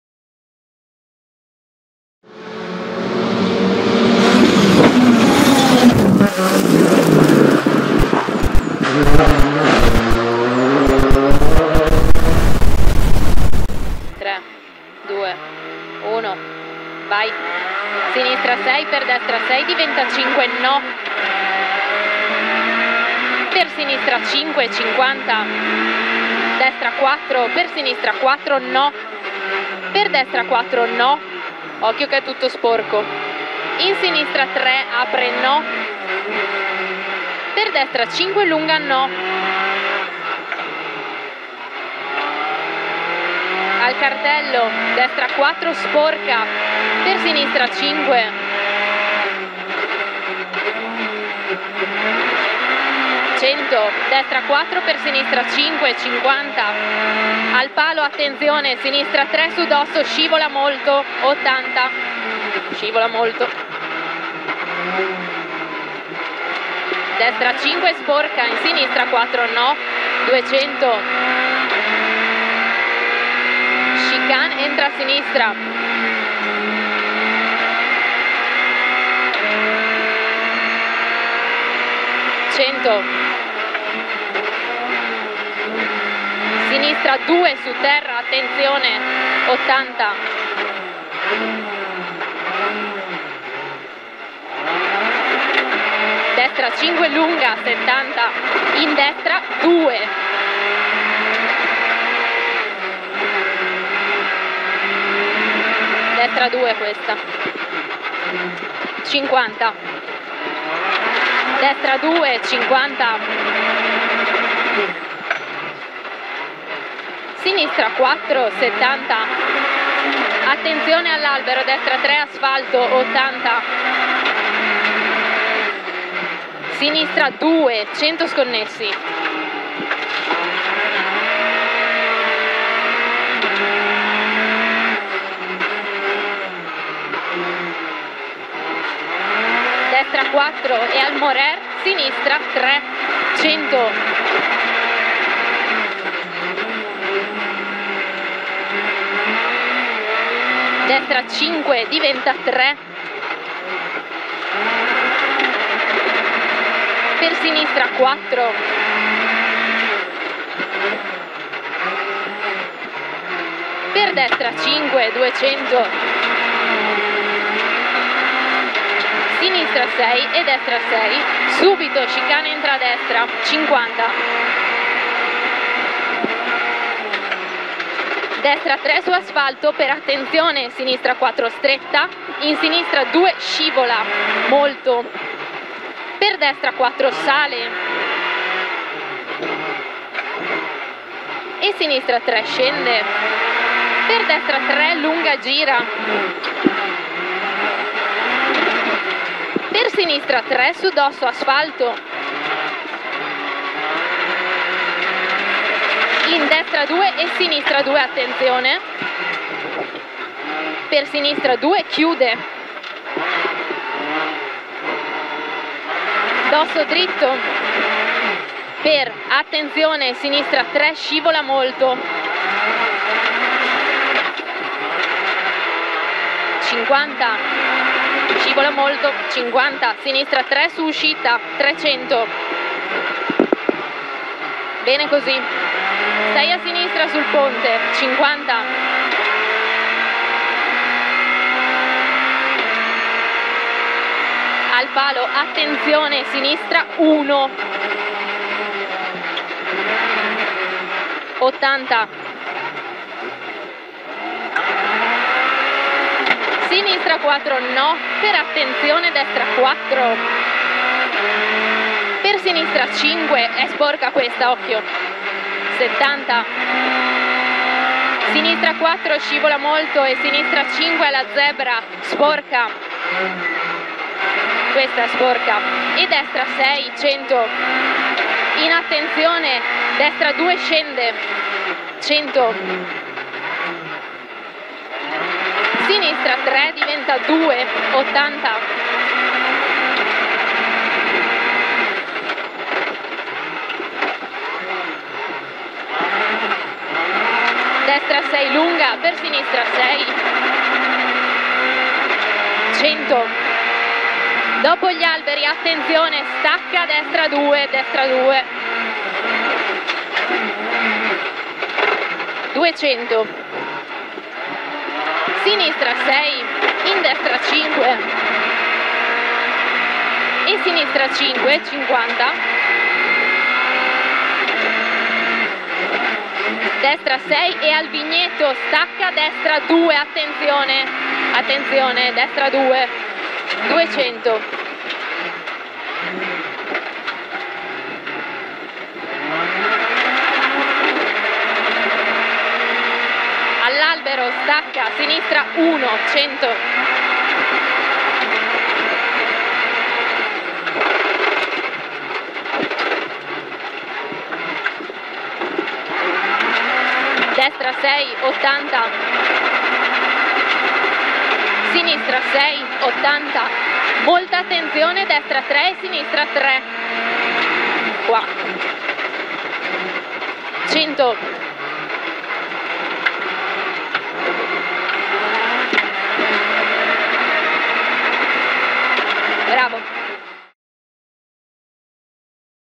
OBC POSSAMAI - MORUZZI // 24° Prealpi Master Show 2023 // P.S.5 San Tiziano
On Board Camera 24° Prealpi Master Show 2023

Possamai Alex
Moruzzi Genny

Peugeot 208 VTi R2B

P.S.5 San Tiziano